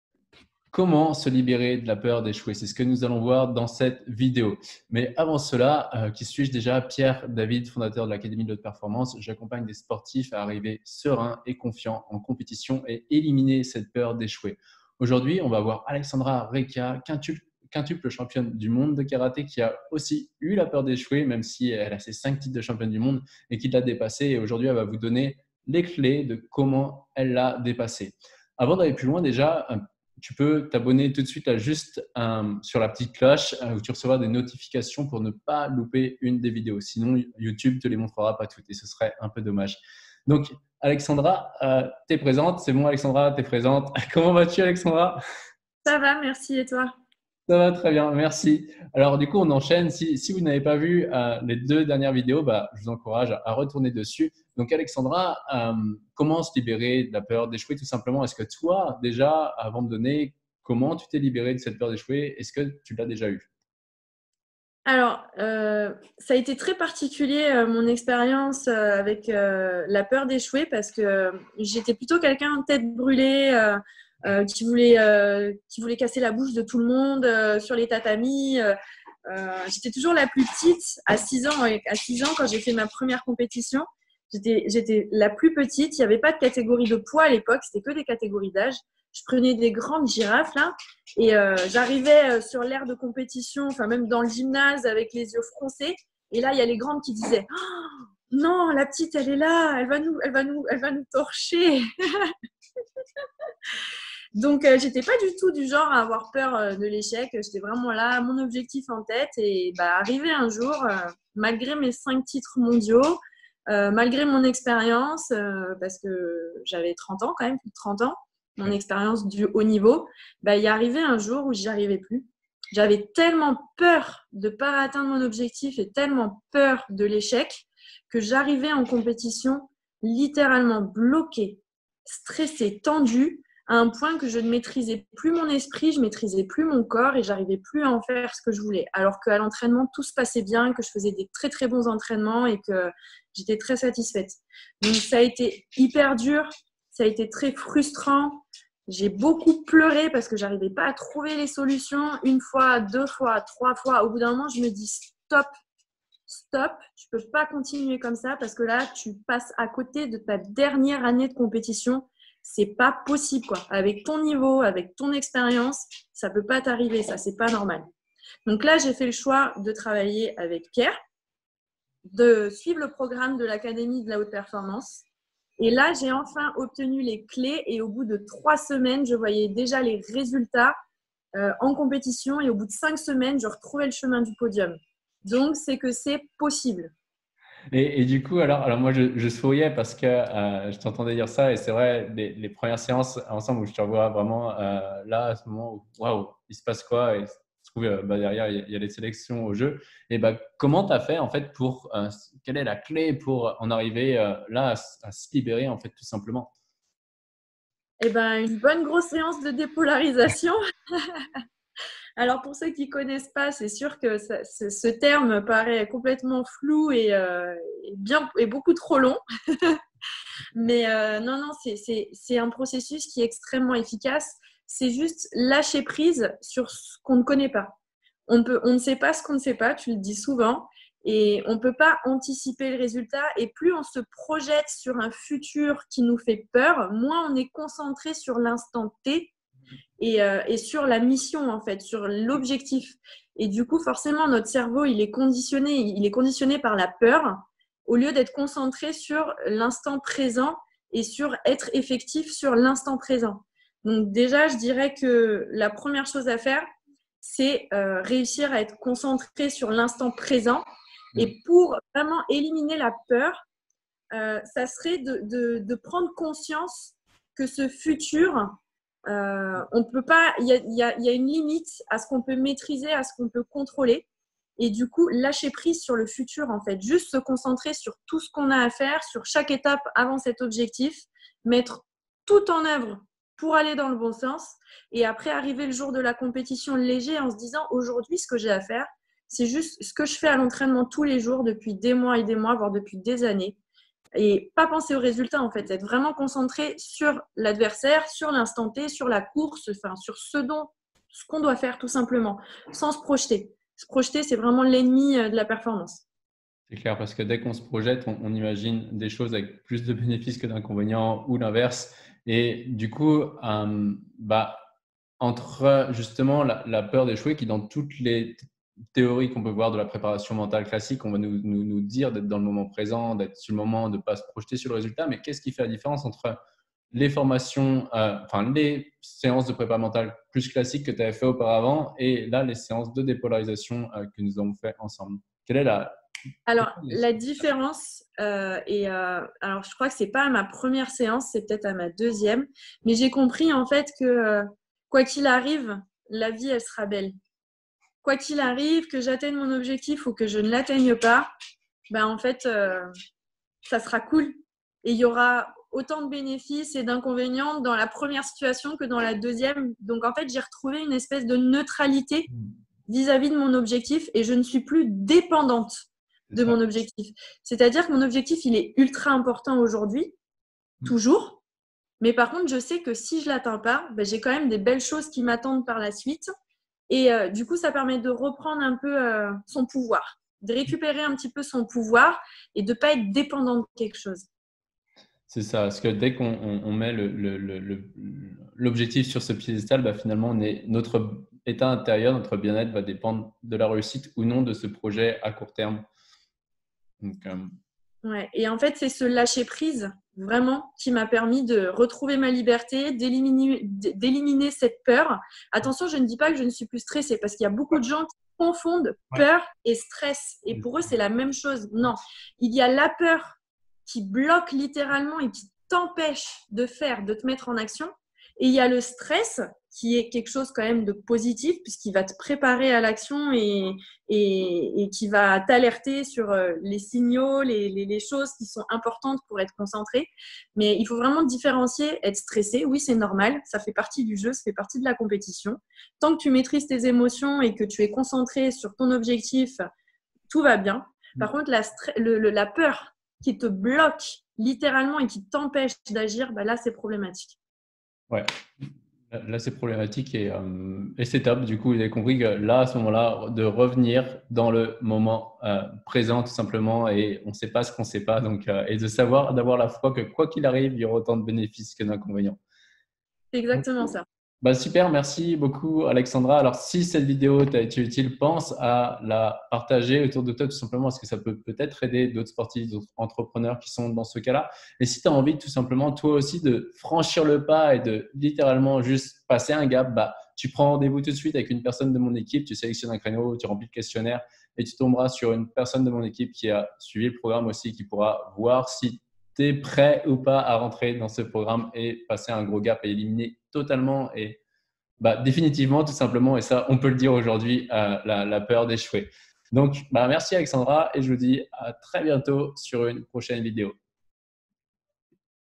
Se libérer de la peur d'échouer et de perdre (avec Alexandra RECCHIA) !
Bonjour ! Aujourd’hui, dans cette vidéo qui se présente sous forme d’interview, vous découvrirez Alexandra, une championne de karaté incroyable. Comment se libérer de la peur d'échouer et de perdre ? Découvrez maintenant ces conseils dans cette vidéo !

J’accompagne des sportifs à être plus sereins et confiants lors des compétitions les plus importantes à leurs yeux.

Aujourd’hui, Alexandra Recchia, championne de karaté, vous livre les clés pour se libérer de la peur d'échouer et de perdre !

La première chose que l’on apprend, et que l’on explique de manière plus détaillée tout au long de cette vidéo, réside dans le fait de ne pas avoir peur de perdre une compétition.

Lorsque l'on subit une défaite sportive, on néglige souvent le processus mental qui suit cette défaite et qui va pourtant nous permettre de rebondir vers la victoire. En effet, il ne suffit pas de se dire que l'on fera mieux la prochaine fois, il faut réellement se poser les bonnes questions. Ces étapes s'intègre dans la préparation mentale qui est fondamentale en sport !

La première étape à suivre est d’accepter sa défaite. Il faut arriver à faire le deuil de cette victoire perdue afin de passer à autre chose et être plus fort.

Il faut également pouvoir tirer un bilan de cette défaite sportive et voir les bénéfices que vous pouvez en tirer. Même dans la perte de quelque chose, on évolue car on en apprend plus sur soi et sur les moyens à mettre en place pour gagner à l’avenir ! Mais je ne vous en dis pas plus et je vous laisse découvrir ces conseils qui vont vous libérer de la peur d'échouer et de perdre.

Pierre